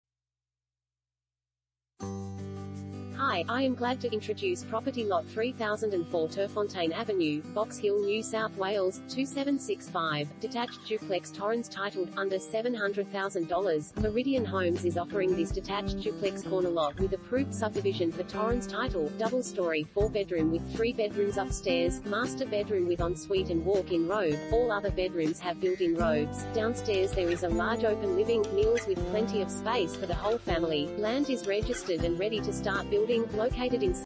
Lot 3004 Turffontein Avenue, Box Hill NSW 2765 | AuPropertyReport.Com
Lot 3004 Turffontein Avenue, Box Hill NSW 2765
Property Type: house
Bedrooms: 4
Bathrooms: 4
Car Space: 1
DETACHED DUPLEX TORRENS TITLED - UNDER $700K